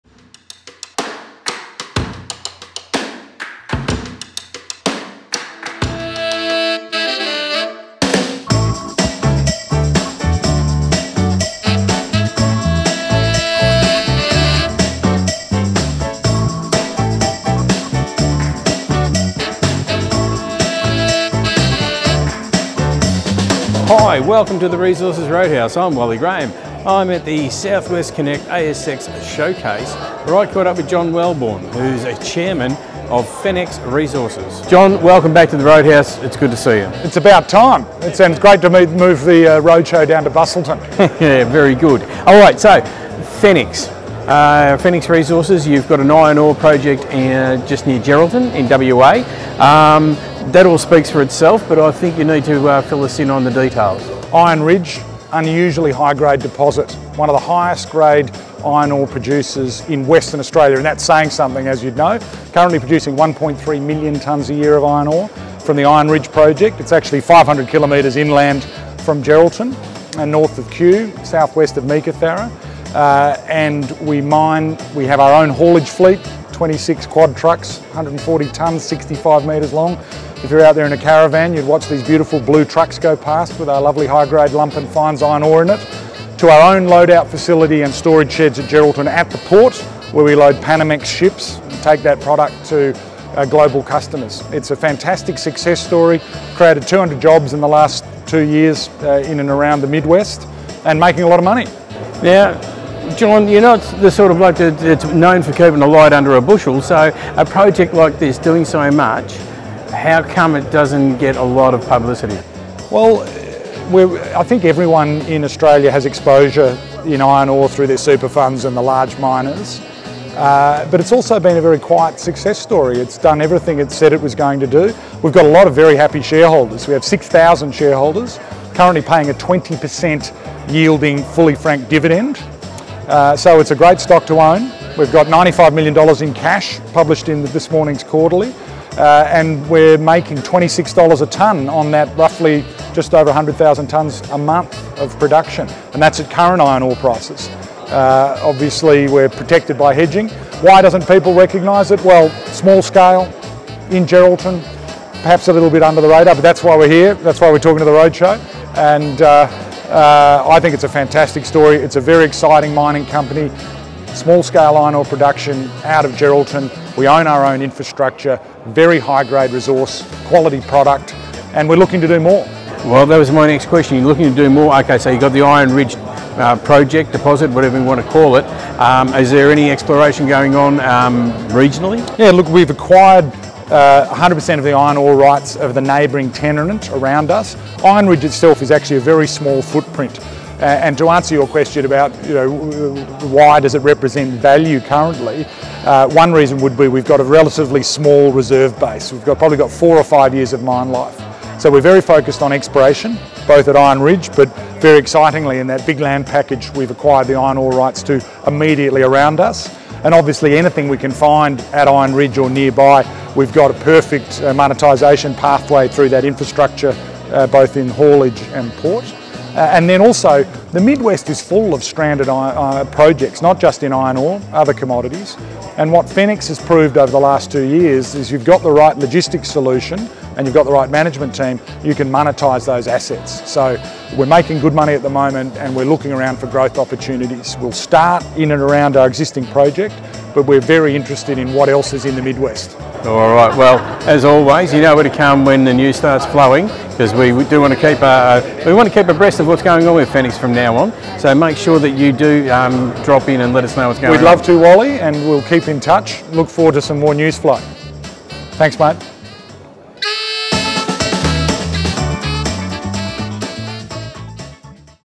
Fenix Resources (ASX: FEX) chairman John Welborn at South-West Connect Showcase.
Fenix Resources (ASX: FEX) has an iron ore project of considerable size and profit making near Geraldton in Western Australia.
Fenix chairman John Welborn provided Wally Grahan of The Resources Roadhouse with some insights to the project while attending the South-West Connect Showcase.